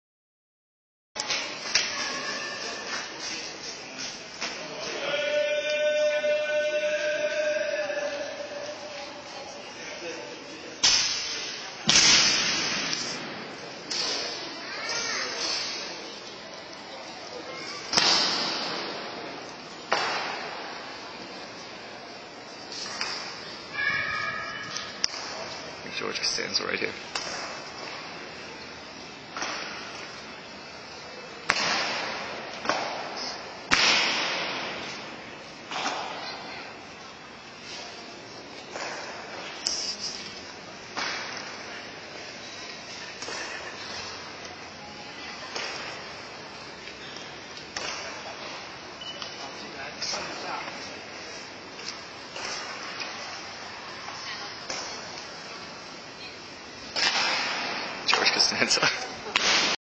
I bump into a famous actor at Sun Yet San Memorial hall
otherwise, this is a pretty lame fuckin video